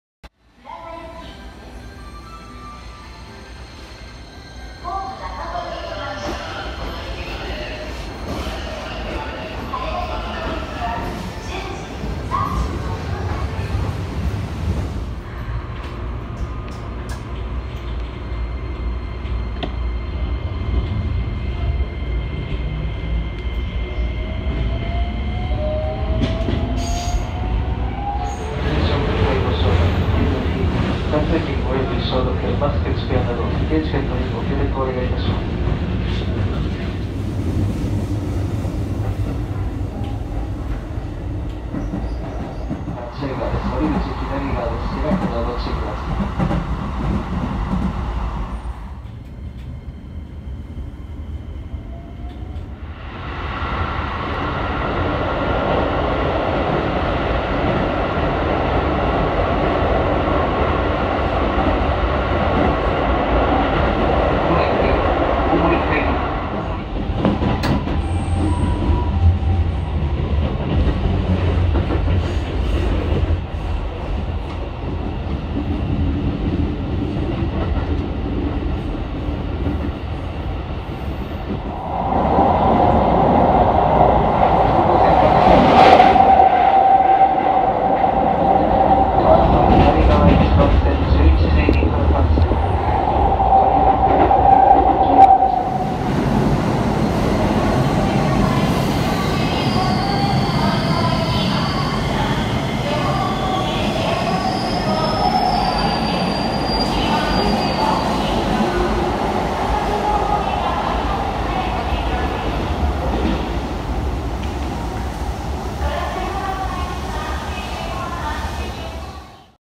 【プチ乗車記#35】京急1607F
京急1607Fのプチ乗車記です。
少しレアなグループの京急1000形での旅をダイジェストにどうぞ！